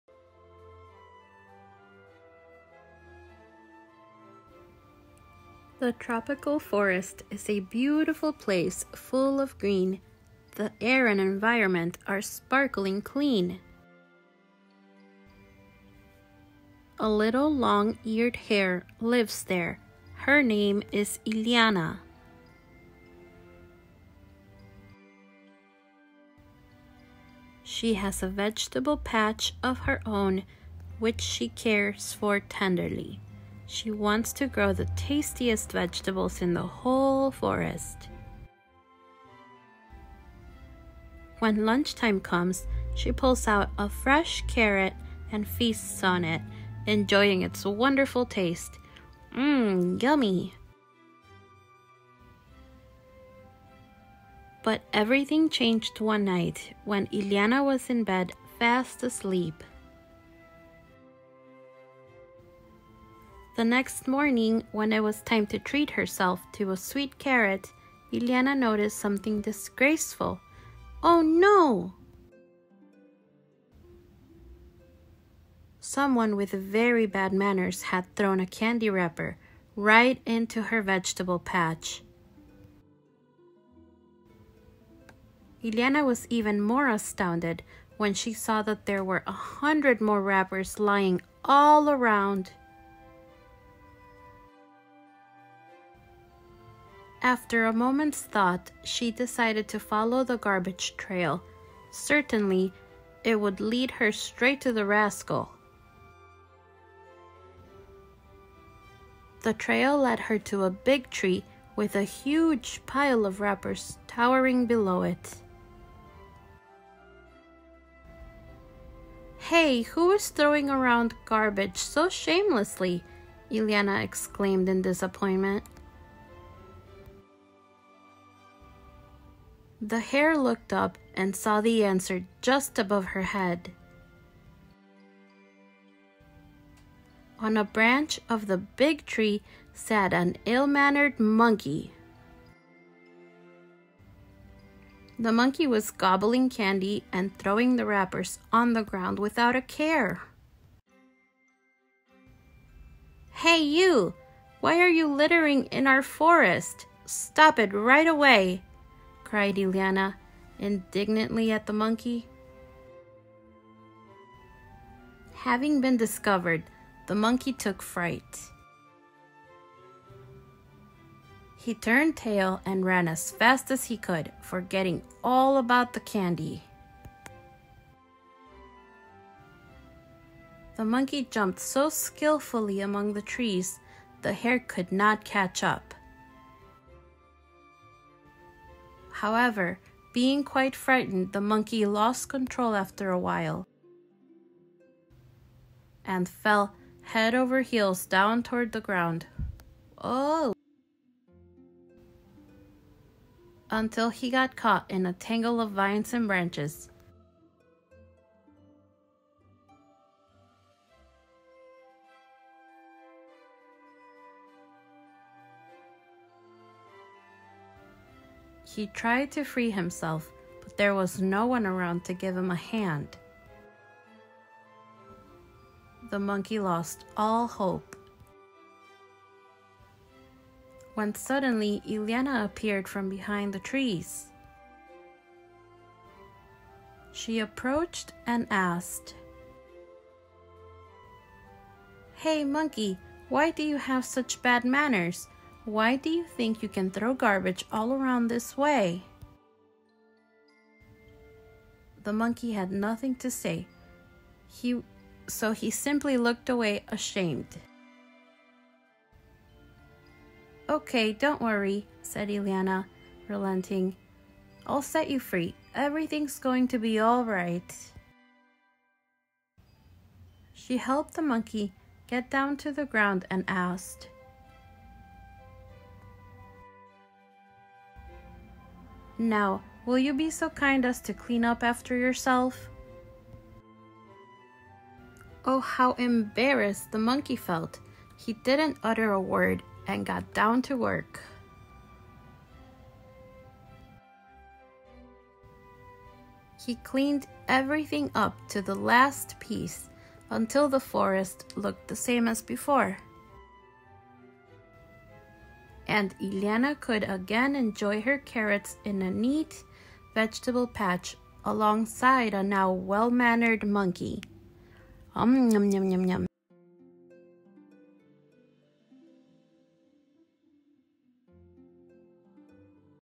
December 5, 2019
A Monkey Manners Tale, read by Xareny, dedicated to Iliana